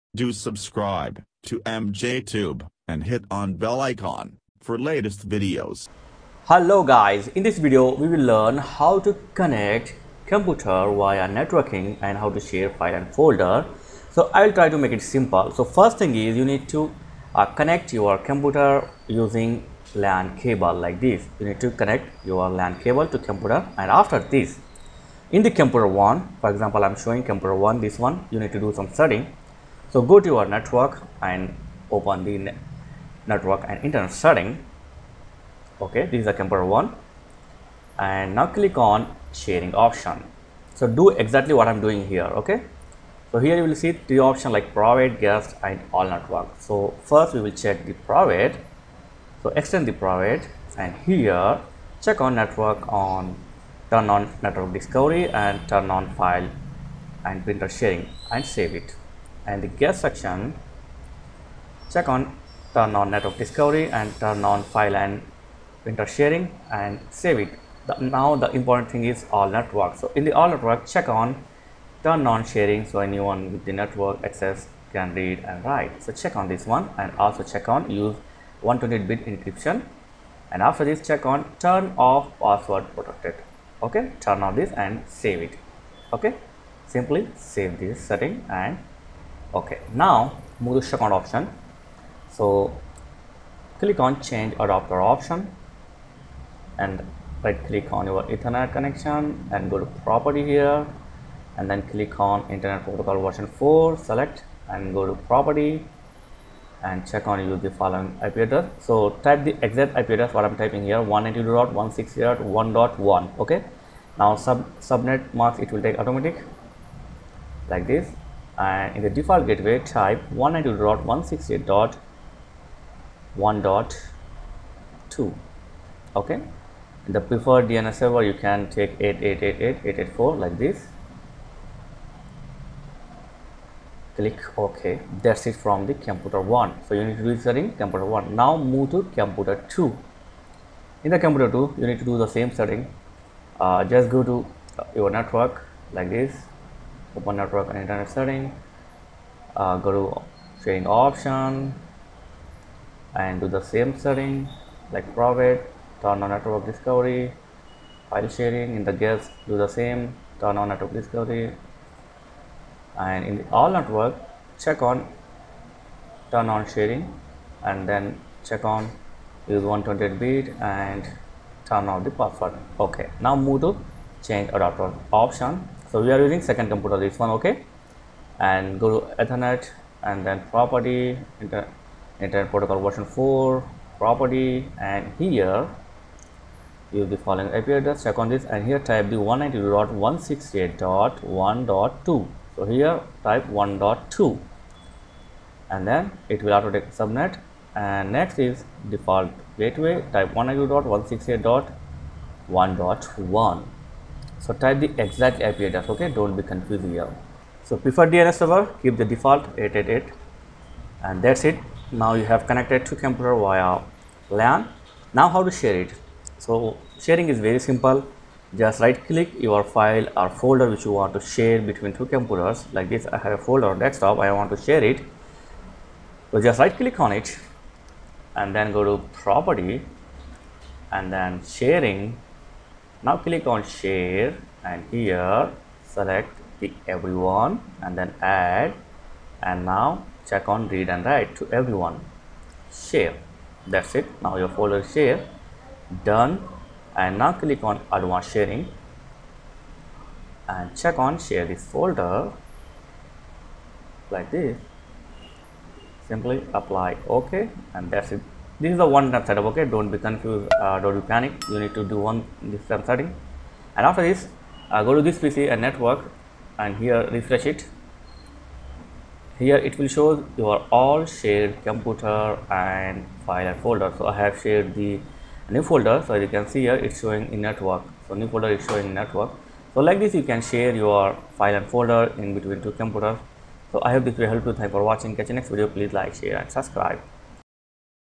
How to Connect Two Computers Via Networking & Share File, Folder & Printer Windows 10
Connect Multiple Computer & Share File & Folder